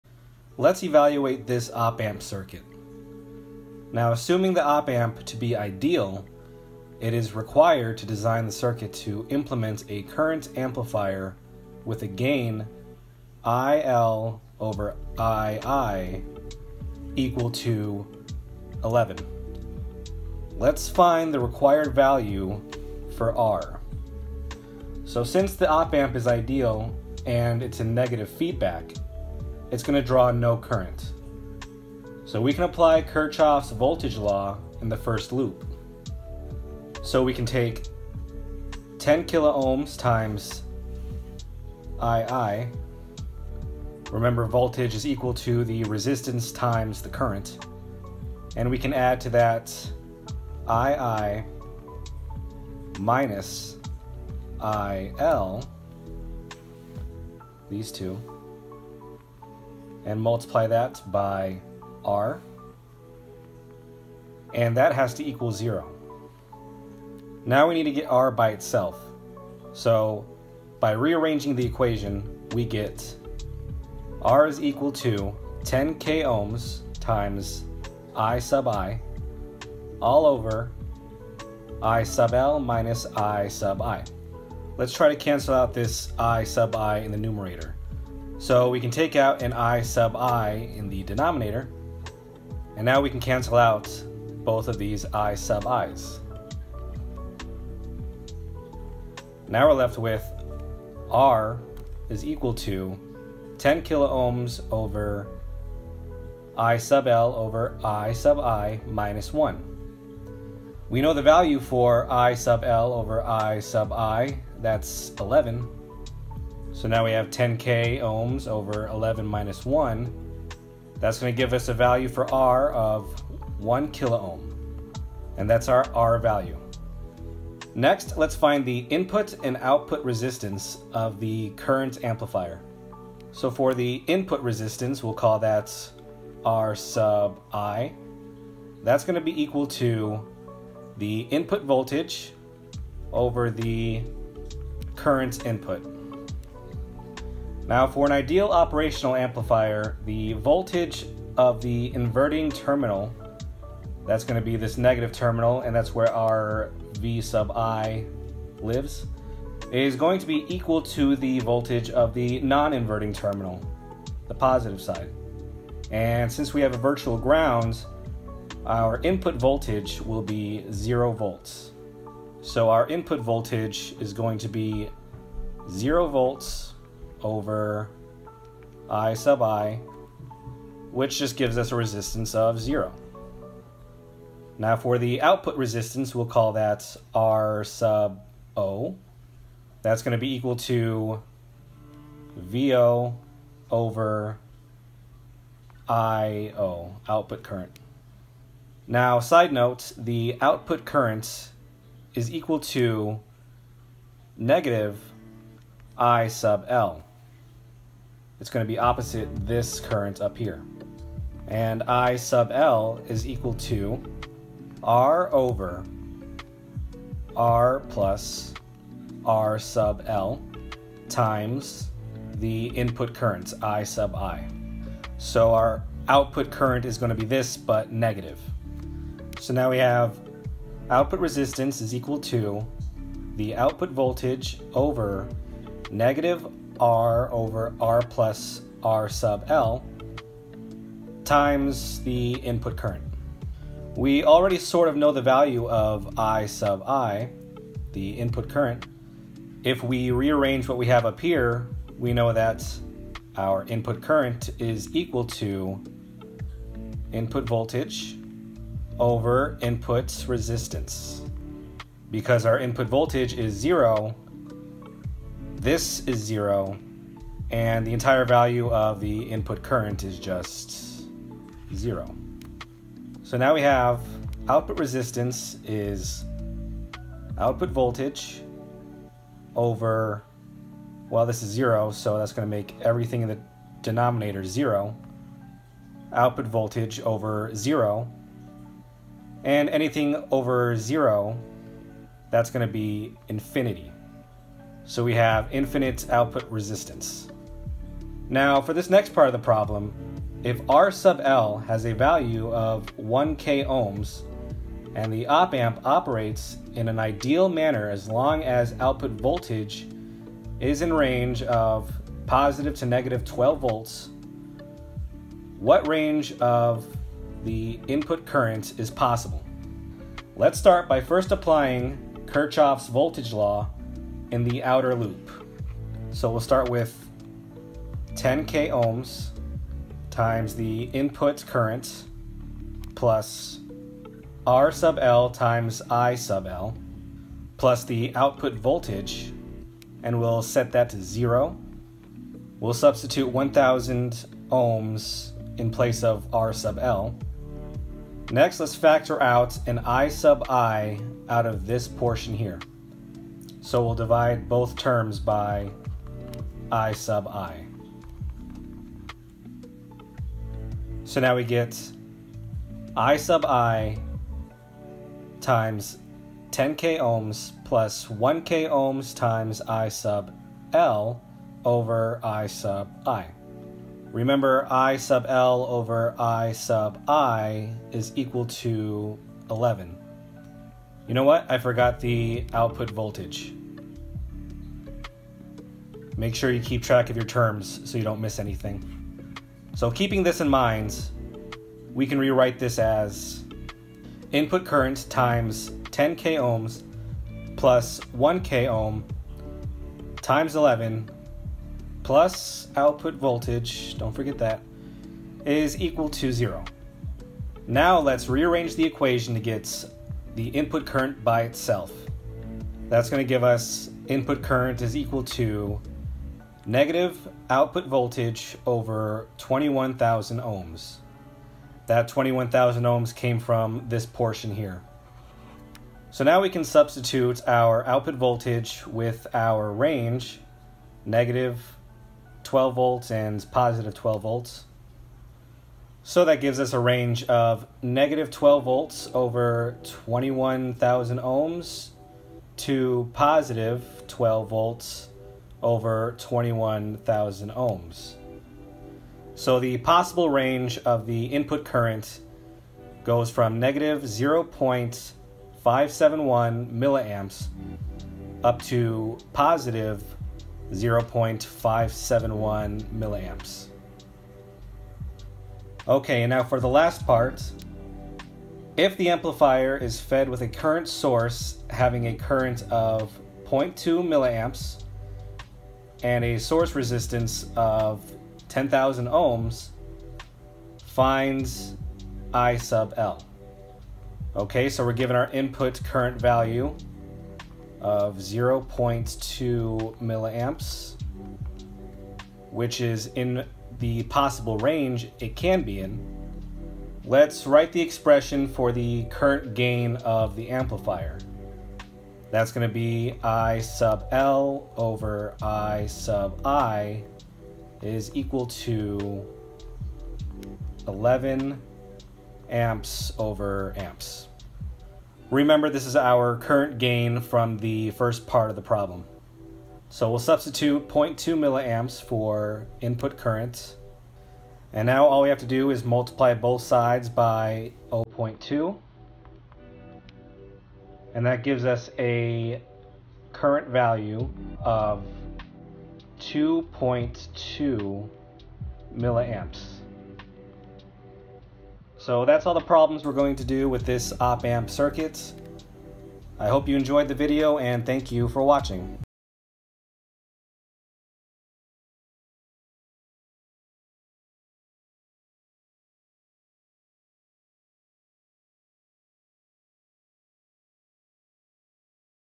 Operational Amplifiers | Problem Set
In this video, find the required resistance value, the input and output resistances, the possible range for the input current, and more.